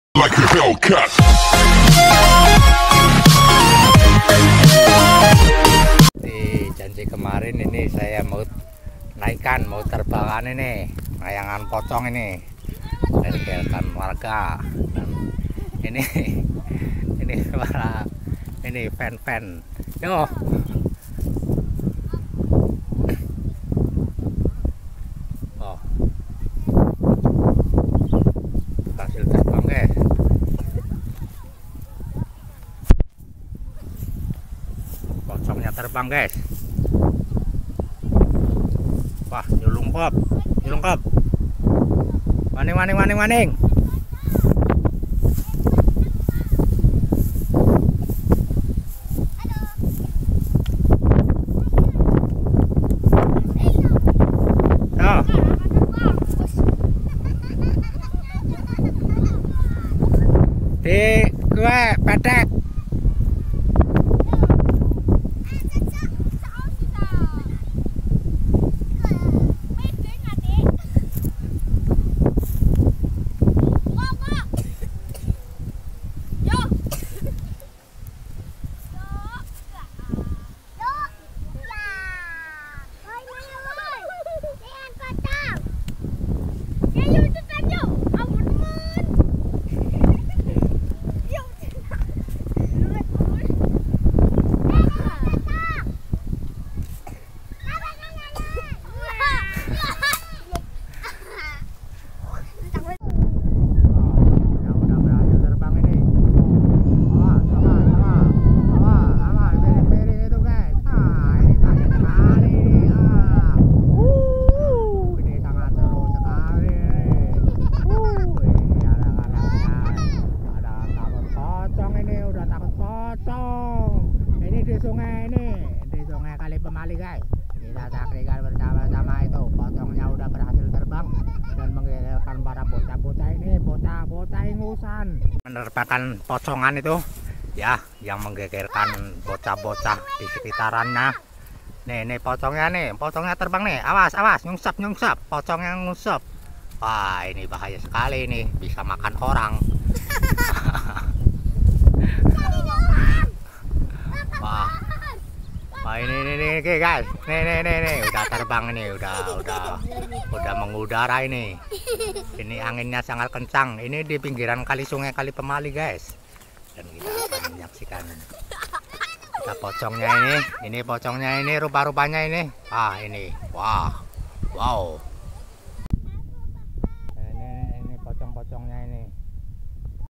Naikin Layangan Pocong Gegerkan Bocah Di sekitar Nya
Assalamualaikum selamat datang kembali gaees seperti janji saya kemarin yang dividio cara bikin layangan pocong kali ini saya tes terbang gaes ternyata mantap gaes terimakasih BOsQuee 🙏